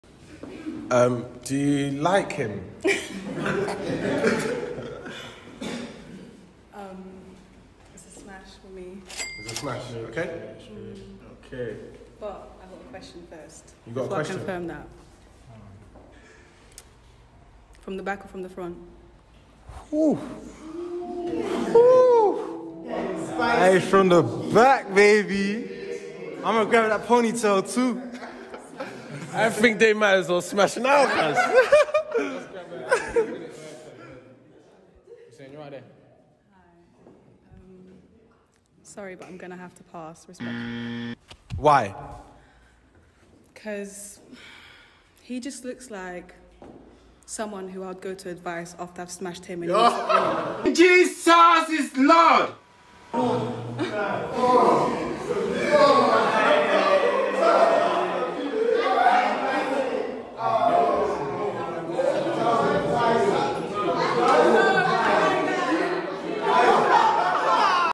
SMASH OR PASS BUT FACE TO FACE UK EDITION | SHE CHOOSING ON TYRONE 😂
King Cid has made smash or pass but face to face a trend on YouTube I found the UK version and thought this clip was funny. Does the black pill have a point?

DO NOT HIT THIS LINK!